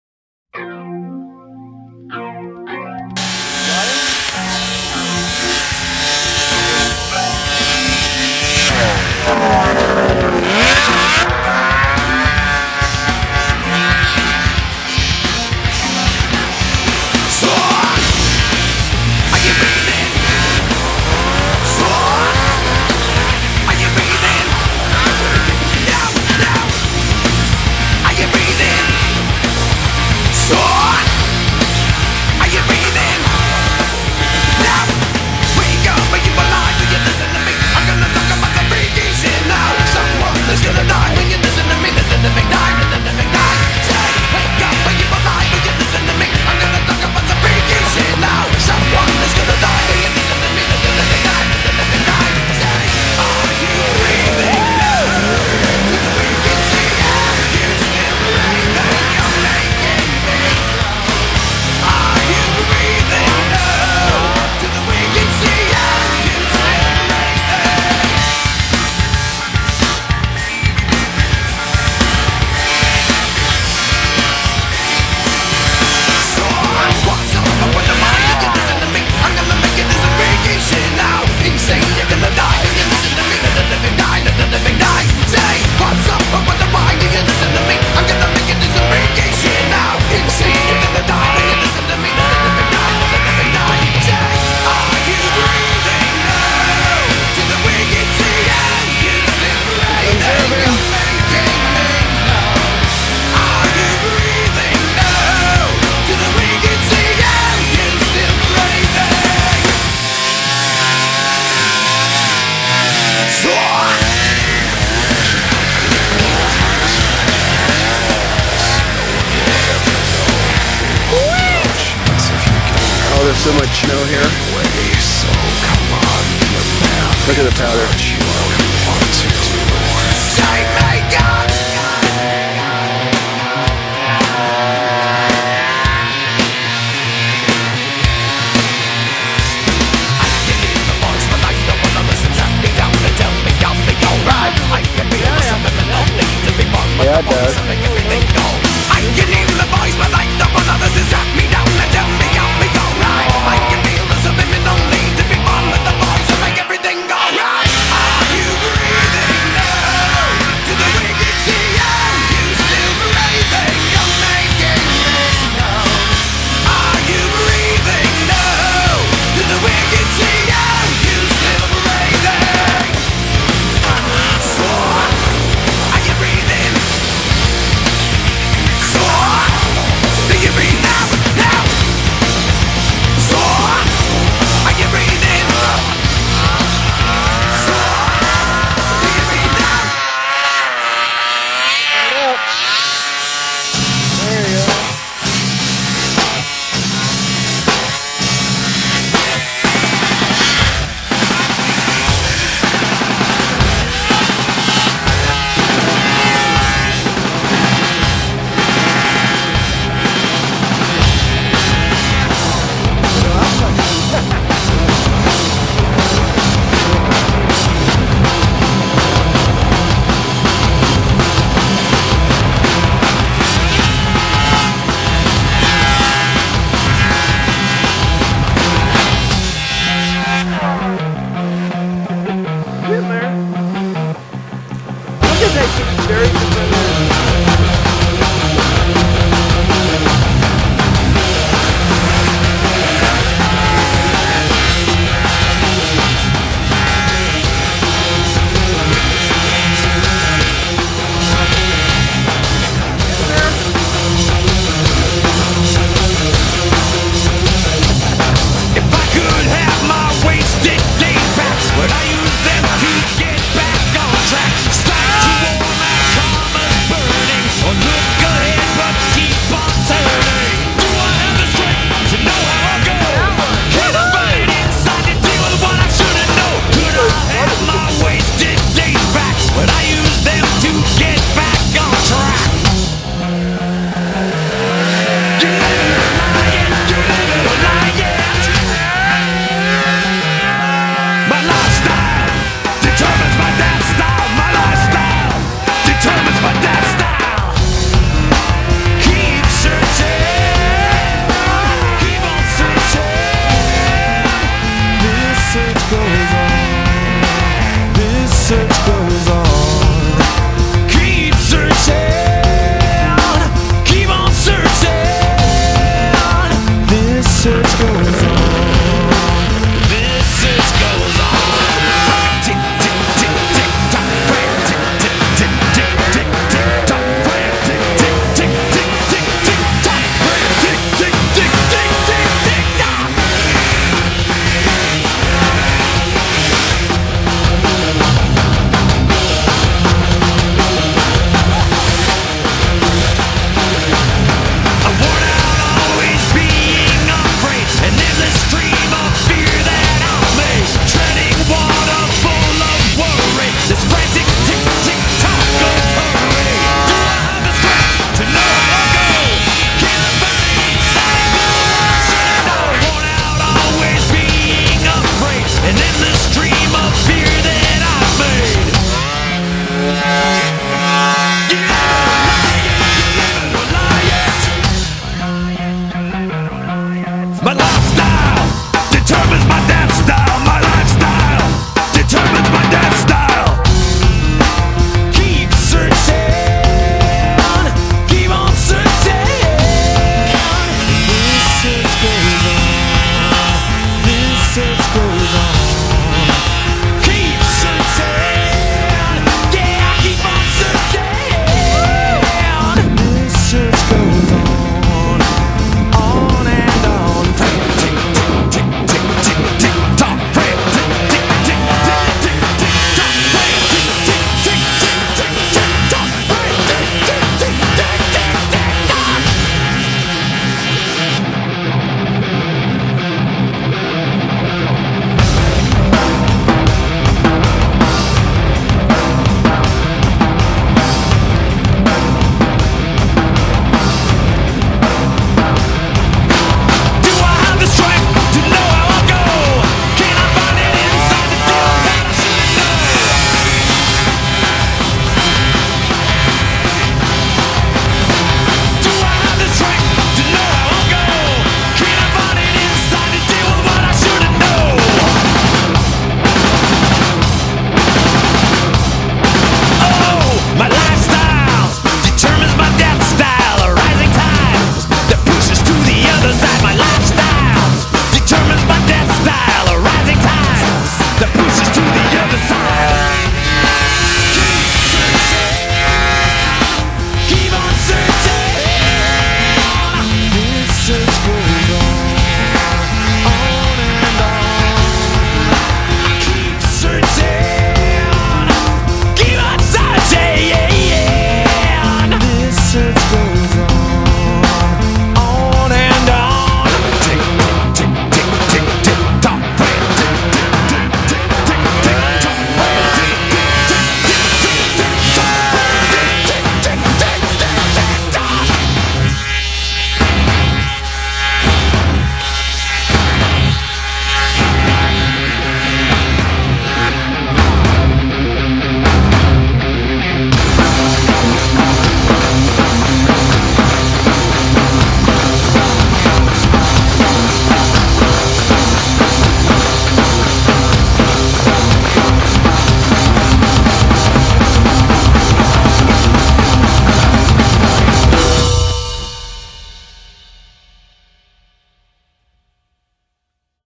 Cle Elum 03-23-2003
ZRT Arctic Cats off the trail in the fresh snow and full throttle hill climbs at the 'rock' - Salmon La Sac - Cle Elum, WA